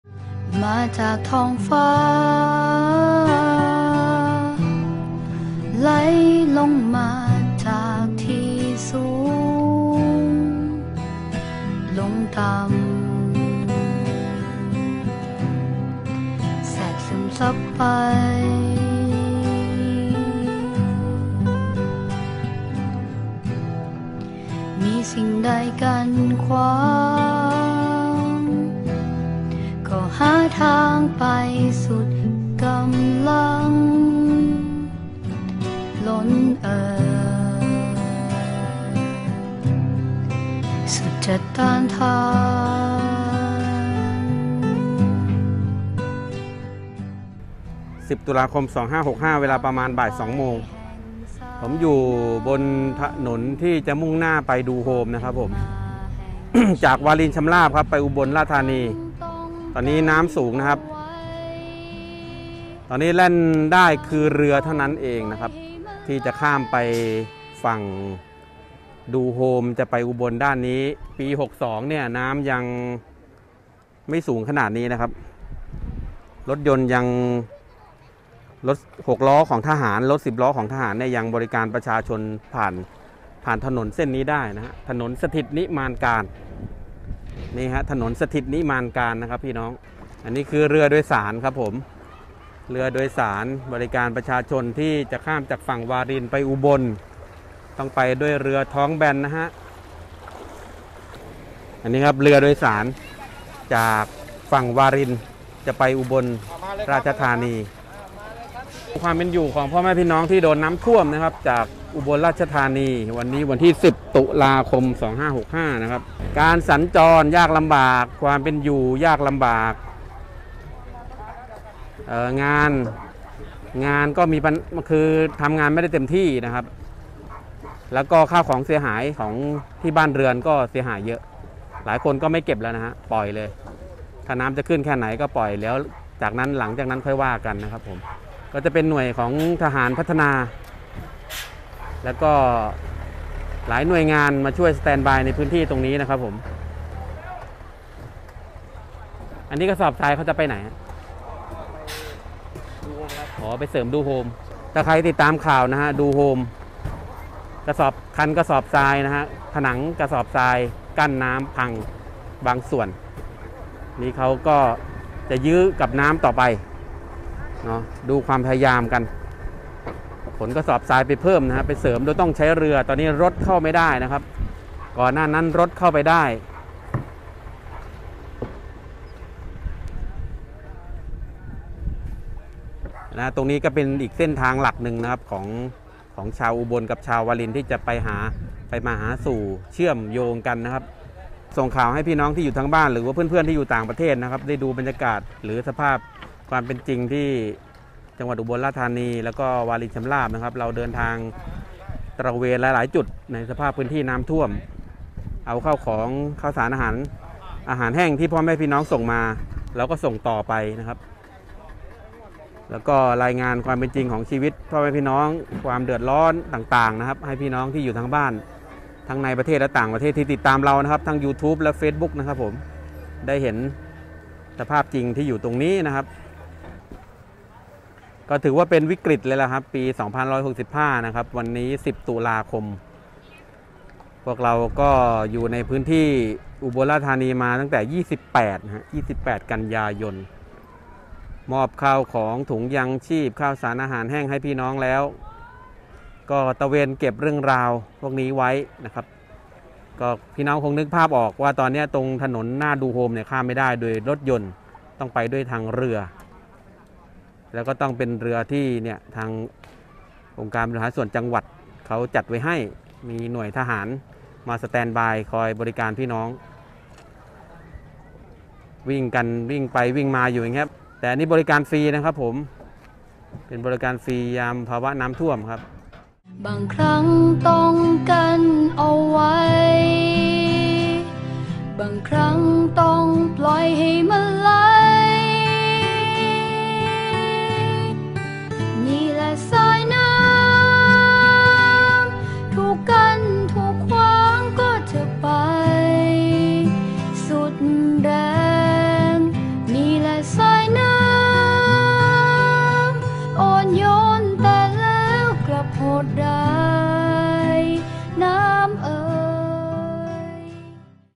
น้ำท่วม..ถนนสถิตนิมานกาล(หน้าดูโฮม) อ.วารินชำราบ จ.อุบลราชธานี//สามอาชีพฯ
น้ำท่วม..ถนนสถิตนิมานกาล(หน้าดูโฮม) อ.วารินชำราบ จ.อุบลราชธานี เวอร์ชันตัดต่อประกอบภาพมุมสูง ภาพบันทึกเมื่อ วันที่ 11 ต.ค. 2565
(ปิดโครงการ..งดรับบริจาค)

1.กสิกรรมไร้สารพิษ(สร้างอาหารที่ปลอดภัยไร้สารพิษ) 2.ปุ๋ยสะอาด(ผลิตปุ๋ยอินทรีย์ที่เป็นมิตรกับคนสัตว์สิ่งแวดล้อม) 3.ขยะวิทยา(จัดการขยะอย่างมีระบบ มีรายได้และลดโลกร้อน)